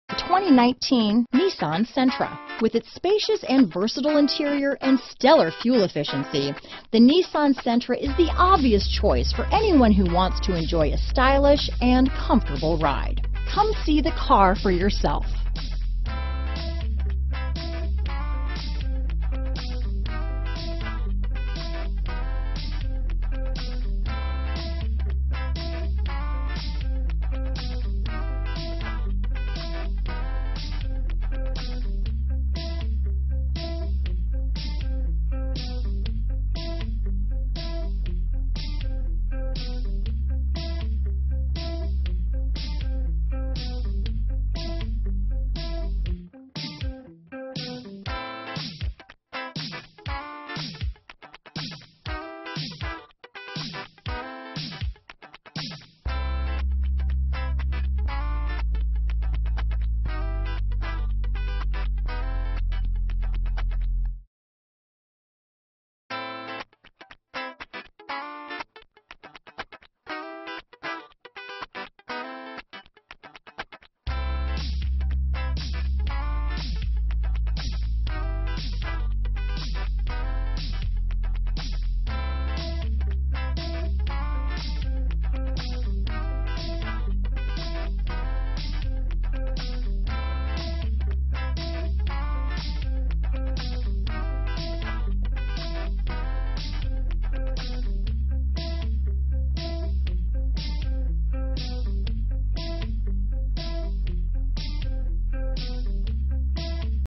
2019 Nissan Sentra Costa Mesa, Huntington Beach, Irvine, San Clamente, Anaheim, CA SE90008
Aspen White New 2019 Nissan Sentra available in Costa Mesa, California at Orange Coast Nissan. Servicing the Huntington Beach, Irvine, San Clamente, Anaheim, CA area.

Aspen White 2019 Nissan Sentra SR FWD CVT 1.8L 4-Cylinder CVT, Charcoal, ABS brakes, Electronic Stability Control, Heated door mirrors, Illuminated entry, Low tire pressure warning, Remote keyless entry, Traction control.Recent Arrival!